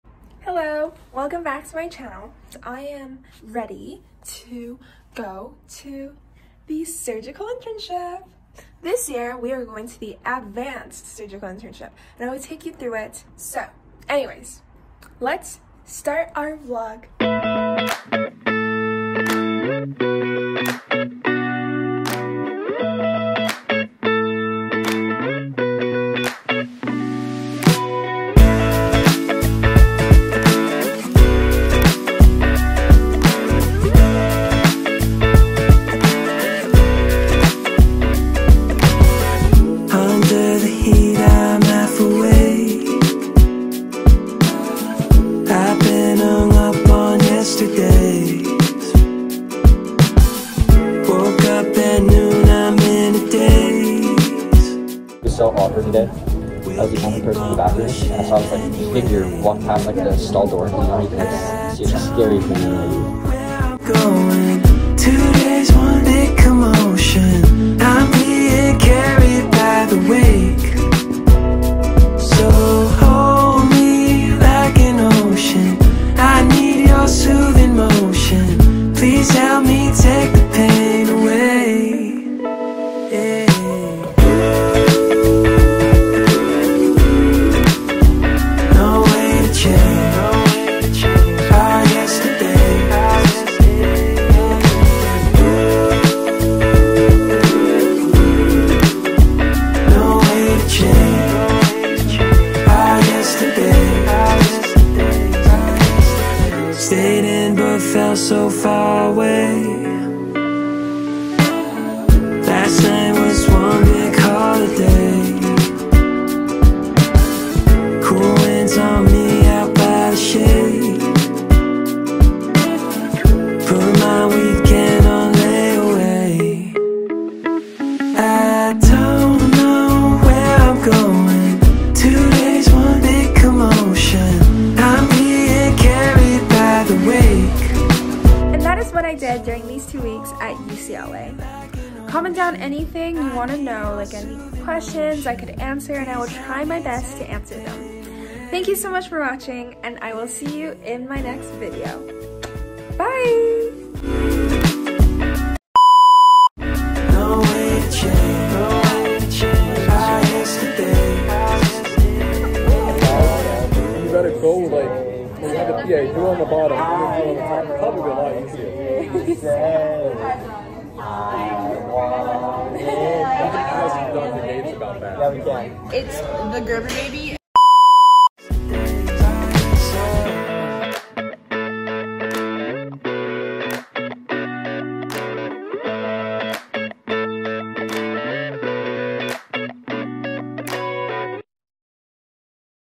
Highschool Surgery Intern at a Leading U.S. Med School
This summer, I went to the advanced cardiothoracic surgical internship. This time, we conducted the Ross, Norwood, Fontan, Atrial & Ventricular Septal Defect Repair Procedures. Let me know if you guys want another video on the explanation of those procedures because I would love to make some! This internship was an extremely enriching experience for me; not only did I learn even more about the heart and bioethics, i was also able to continue career exploration!! Hope you guys enjoy the vlog~